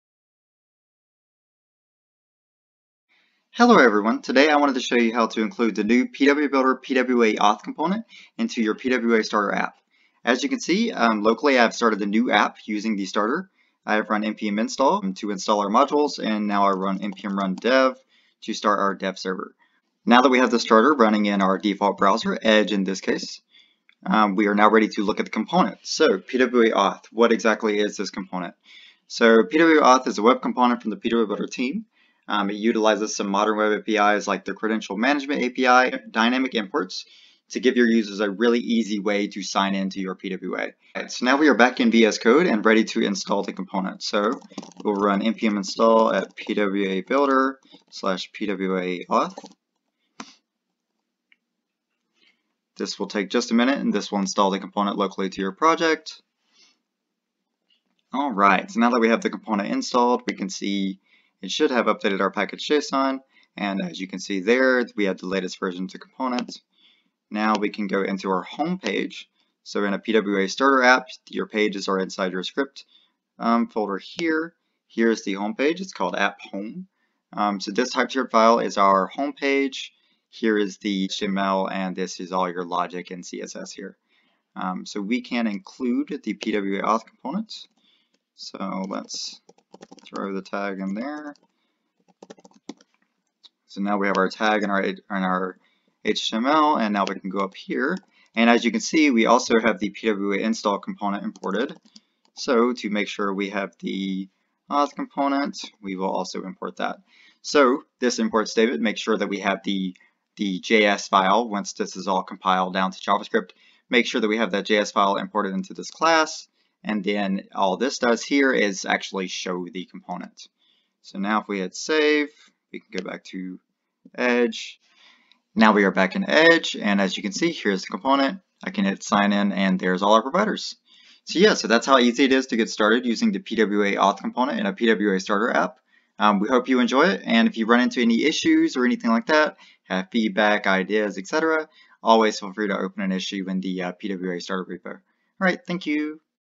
Using the pwa-auth component in a pwa-starter app!
Today we will go through how to add the PWABuilder pwa-auth component to your pwa-starter app!